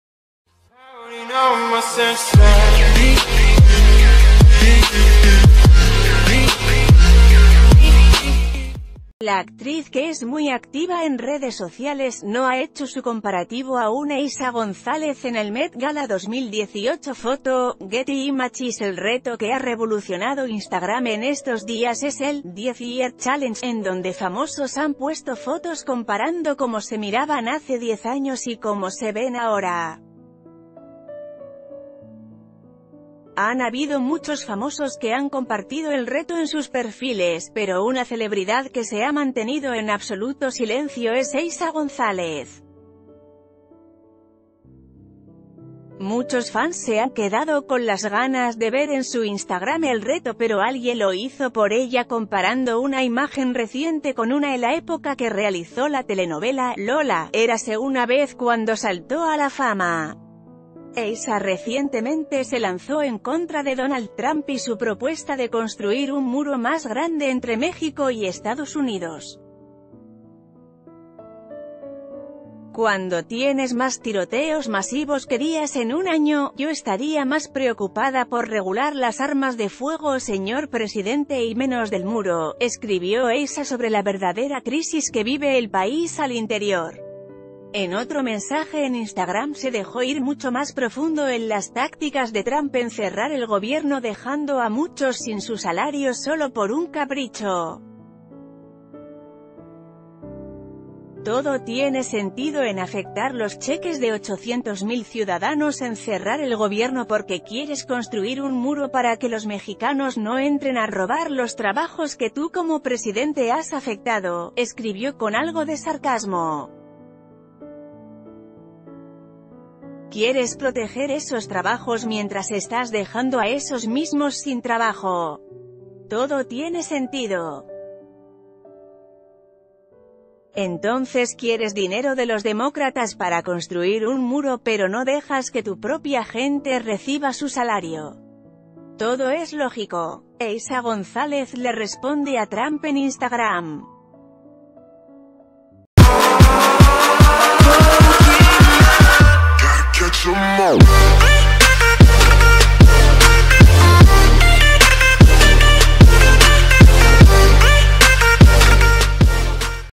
El ’10 Year Challenge’ de Eiza González que no vas a ver en su Instagram
La actriz que es muy activa en redes sociales no ha hecho su comparativo aún Eiza González en el Met Gala 2018 Foto: Getty Images El reto que ha revolucionado Instagram en estos días es el “10 year challenge” en donde famosos han puesto fotos comparando como se miraban hace 10 años y como se ven aho...